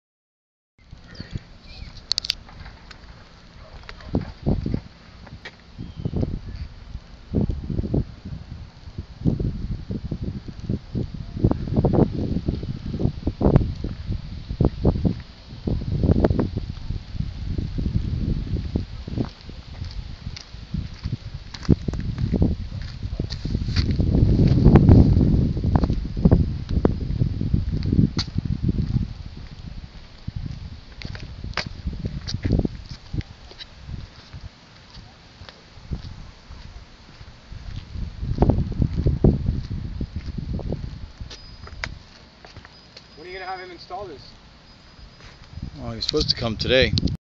PATENTED ARTEZANOS HYBRID SOLAR TIPV/T PART 11 ARTEZANOS COMPLAINT ARTEZANOS REVIEW
Patented Artezanos HYBRID TIPV SYSTEM using high performance SHARKSKIN ULTRA underlayment.